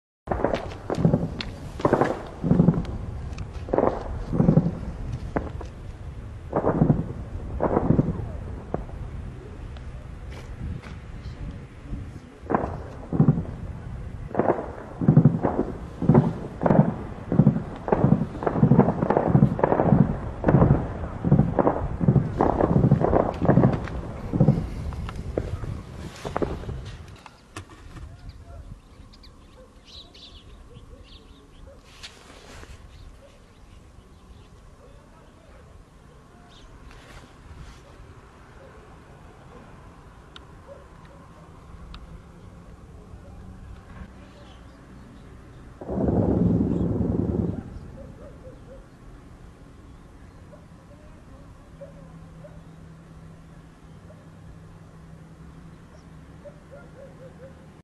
فيديو 2 من الاشتباكات في ناحية عين عيسى بين الجيش التركي والفصائل الجهادية الموالية لها، وقوات سوريا
سوريا الديمقراطية.
الاشتباكات بدأت مساء أمس الخميس، ومستمرة إلى هذه اللحظة.